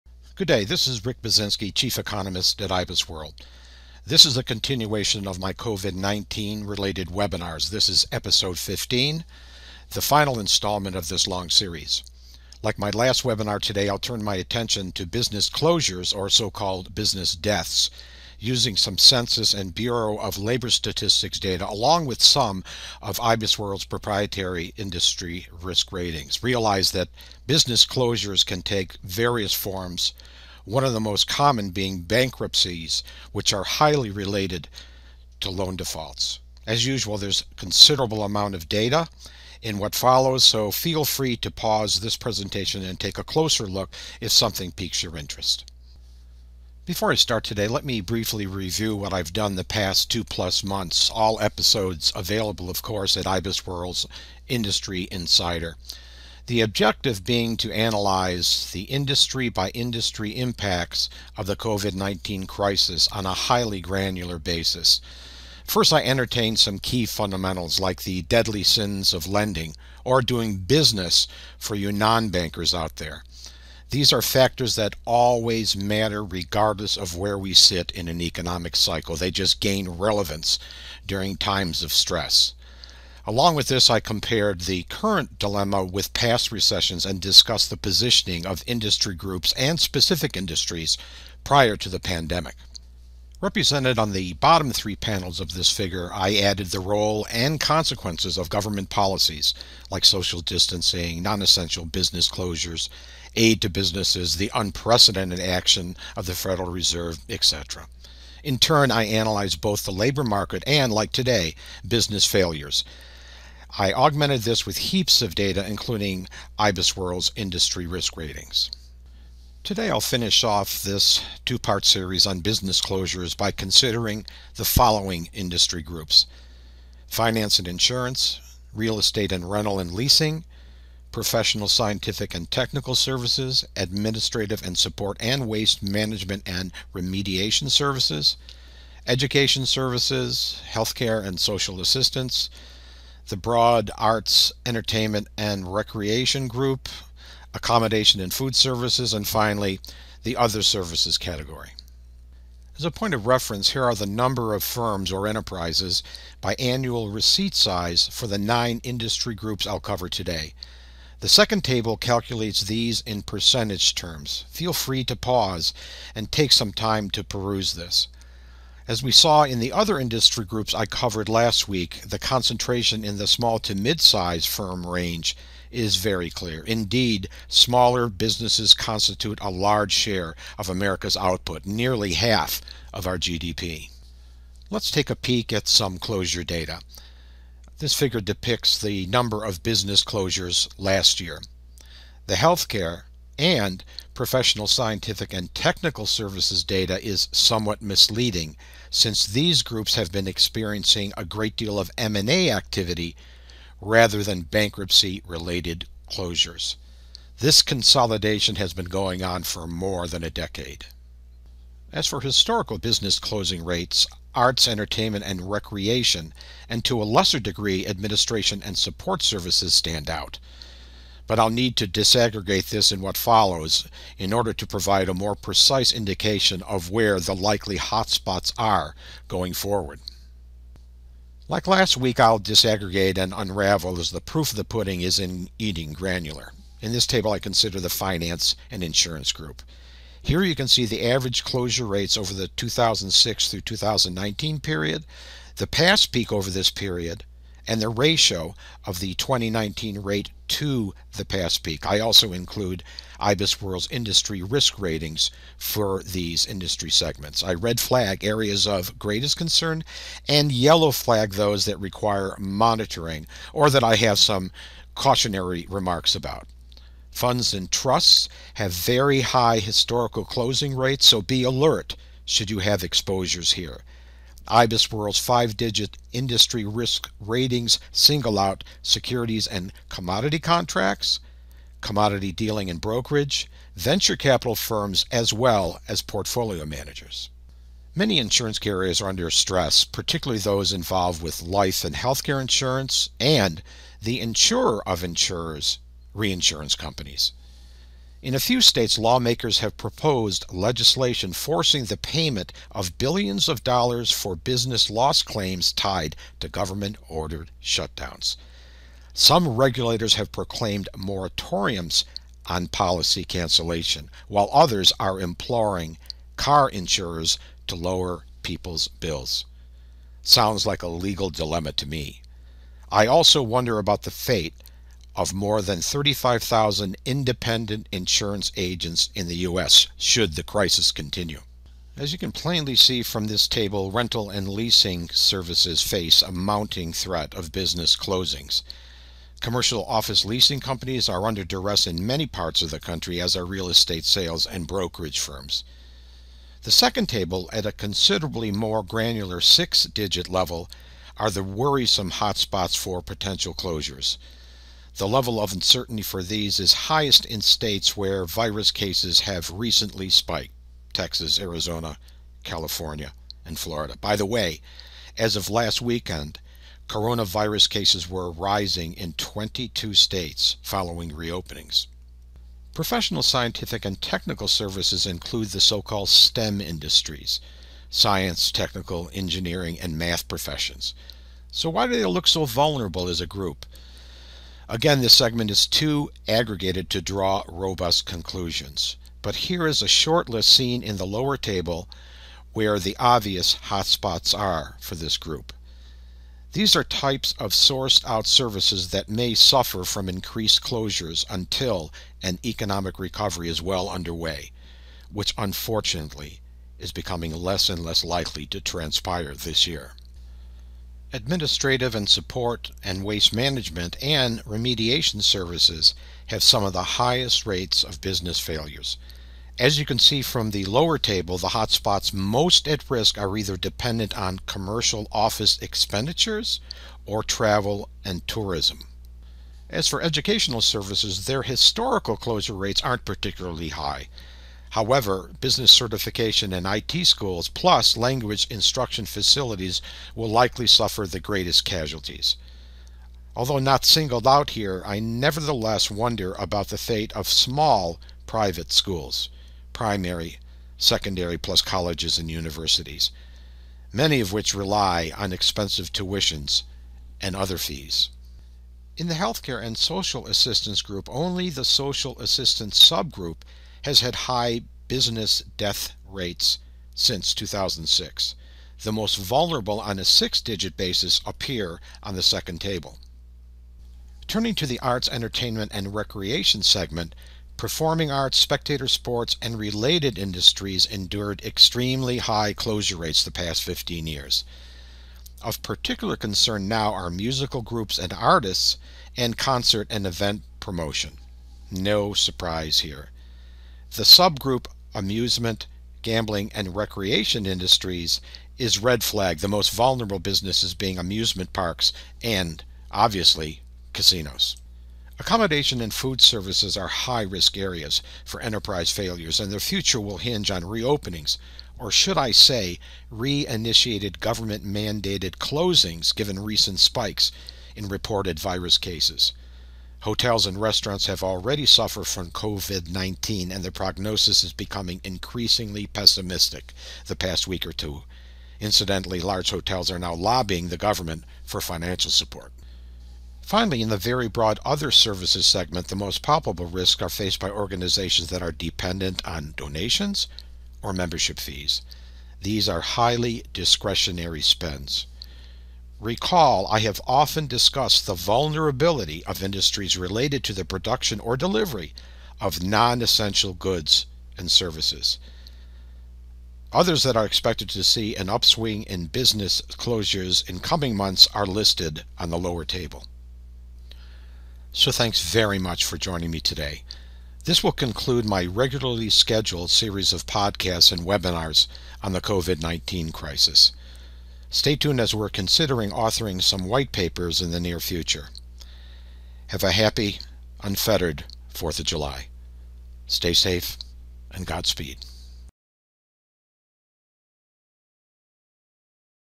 Episode 15: COVID-19 Insights from IBISWorld’s Chief Economist
Rick Buczynski, PhD, continues his discussion of business closures (or deaths) using census and bureau of labor statistics data along with some of IBISWorld’s proprietary industry risk ratings. He'll address: finance & insurance, real estate & leasing, professional/scientific/technical services, educational services, arts & recreation and more.

This will also serve as the finale in this COVID-19 Insights series of webinars.